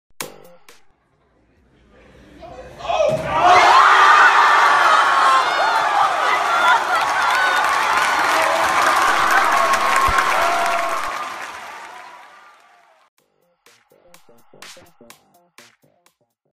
Student nails impossible shot in chemistry class, becomes campus hero
During a lecture, a student sunk a near-impossible shot into a trash can, earning everyone in the class an A on that day's quiz.